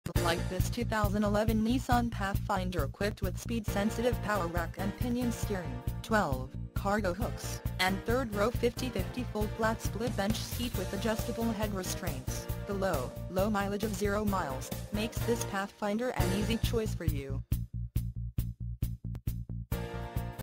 used Nissan Pathfinder Baltimore MD 2011 located in Maryland at LS Lexus
This used Nissan Pathfinder Baltimore 2011 is located in Maryland and is offered by LS Lexus and can be compared to any used Nissan Pathfinder MD: